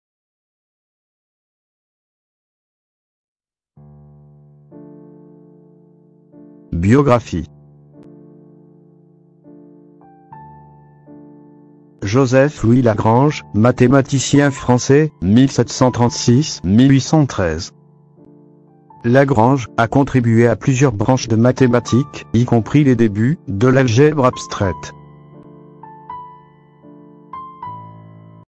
🔴MATHSCLIC SHORTS | BIOGRAPHIE COURTE ~ JOSEPH LOUIS LAGRANGE
👉★Courte biographie du mathématicien français Joseph Louis Lagrange.